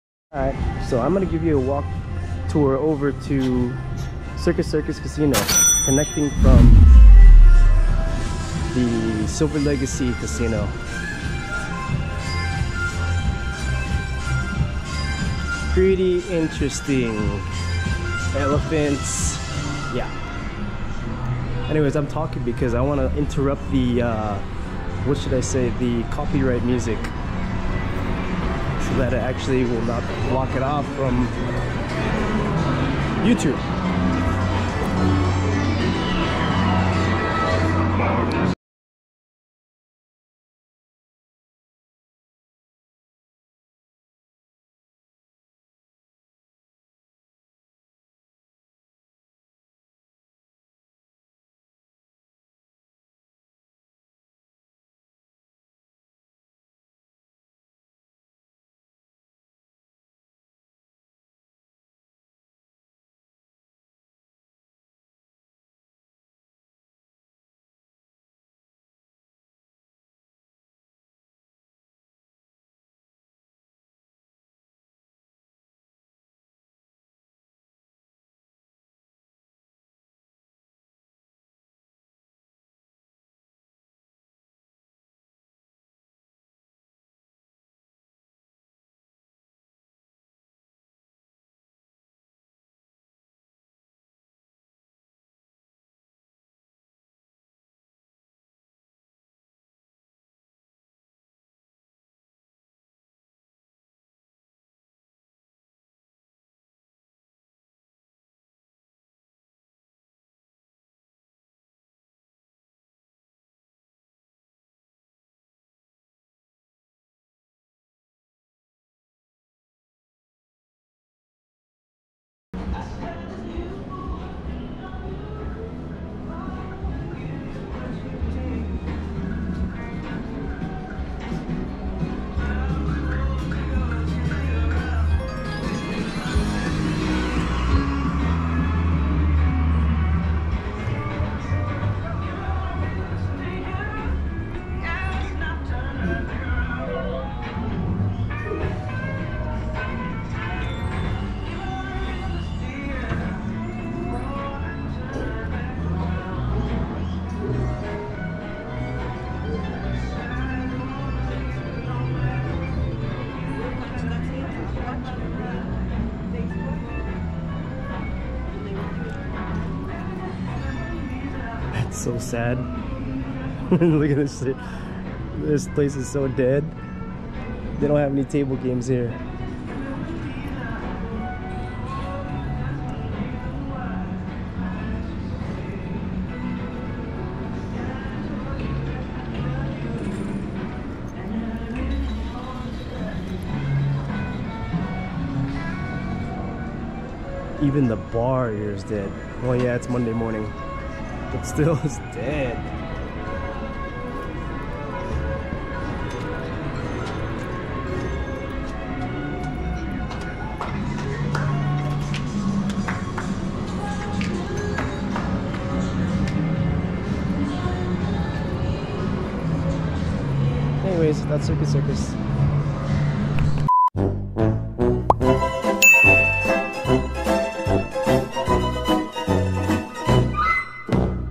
WALKING TOUR of CIRCUS CIRCUS Casino in RENO Nevada
Circus Circus Reno is a hotel and casino located in Downtown Reno, Nevada. It anchors a network of connected hotel-casinos in the downtown Reno core that includes Silver Legacy Reno and Eldorado Reno and are owned and operated by Caesars Entertainment. It includes a 1,620 room hotel and a 66,515 sq ft (6,179.4 m2) casino which features free circus acts on a regular basis throughout the day over the midway which also offers 33 carnival games.

It is the second-largest hotel in downtown Reno (and third-largest in the Reno area overall) by number of rooms.

Previous owners of Circus Circus Reno were Mandalay Resort Group, formerly known as Circus Circus Enterprises (1978–2005) and MGM Resorts International, formerly known as MGM Mirage (2005–2015).
- Wikipedia

Let me know if you would like another casino walk through?